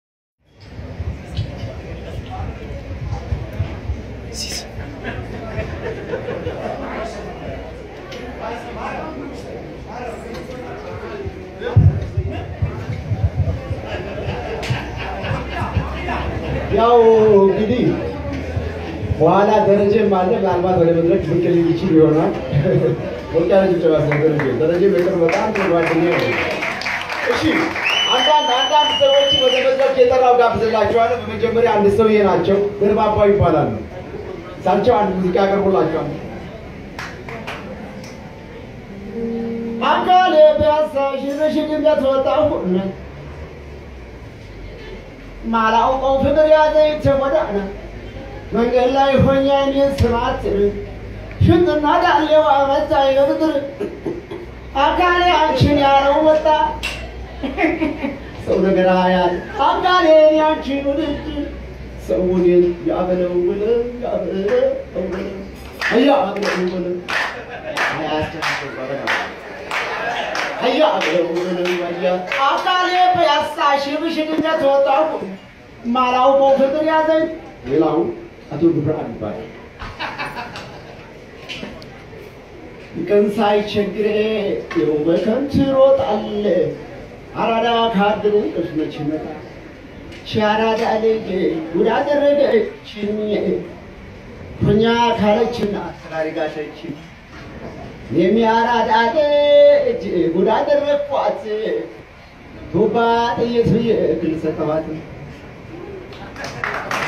Ethiopia: Comedian Limeneh Tadesse after almost 20 years (ቀልደኛው ልመነህ ታደሰ ከ ዓመታት በኋላ)
Ethiopia: Comedian Limeneh Tadesse after almost 20 years at Eset Bar & Restaurant in Addis Ababa, Ethiopia
(ቀልደኛው ልመነህ ታደሰ ከ ዓመታት በኋላ በእሴት ባርና ሬስቶራንት አዲስ አበባ፣ ኢትዮጵያ)